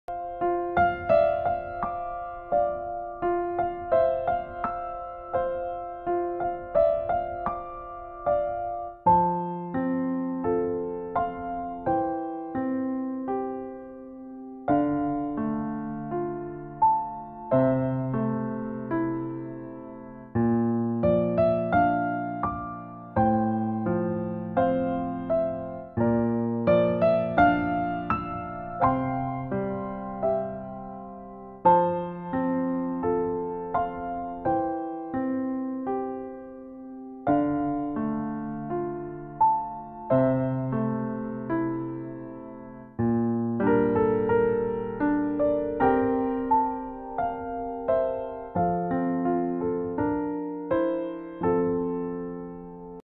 Cute Nerite snail mopping up the aquarium wall - Close up algae eater
Nerite snail will not be able to reproduce in freshwater. It can survive just fine in it though. Therefore this snail is one of the best algae eater for your aquascape.